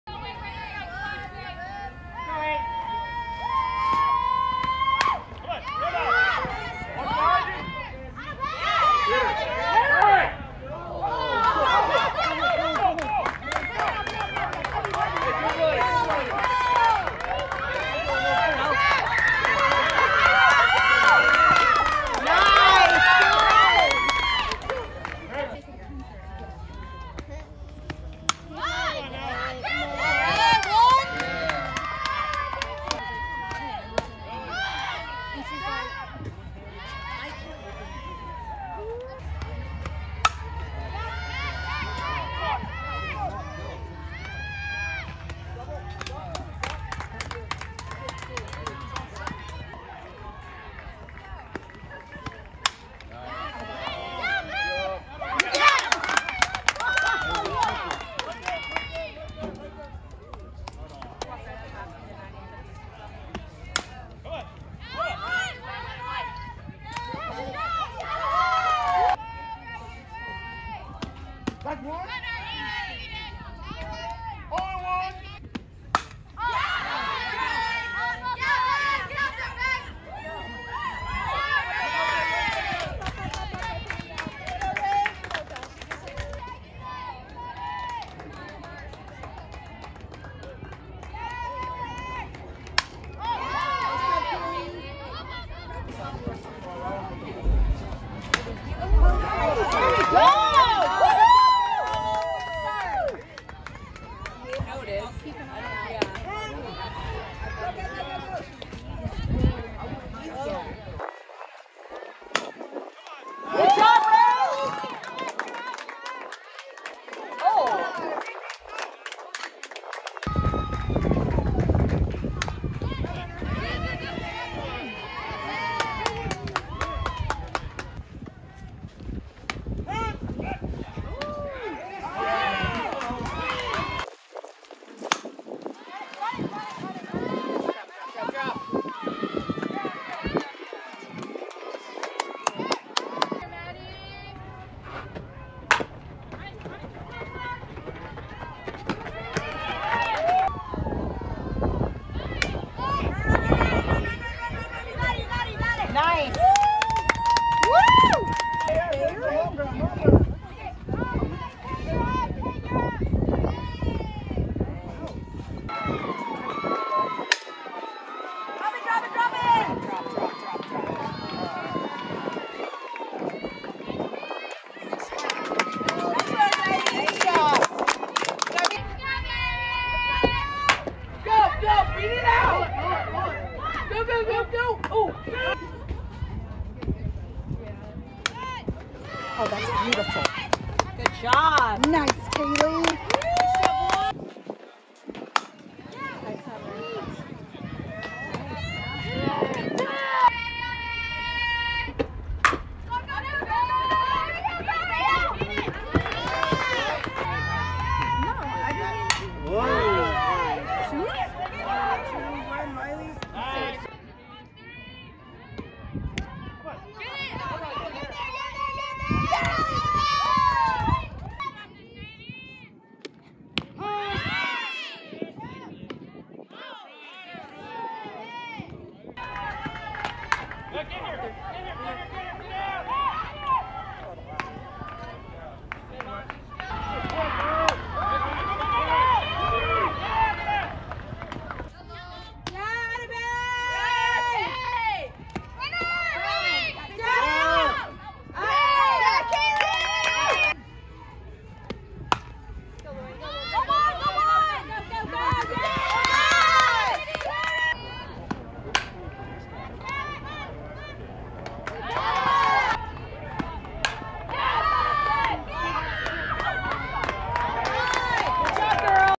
Kayli Clark - 2024 MI/OF/Speed
Class of 2024 Kayli Clark at the 2022 Triple Crown Work Series in Reno.   SS/2ND/OF/Speed.
Kayli was .533 BA .562 OBO 2 RBI 7 SB 8 R 1 PO 7 A for 3 days of play.

Home to Home - 11.91
Arm Speed - 61 mph

Ball Speed off the bat - 68 mph

Broad - 6’9”    Vertical -19.5”
GPA - 3.8

Field of Study - Business

Short Stop, Center Field, Outfield, Middle Infield, Softball, Slapper

Softball Class of 2024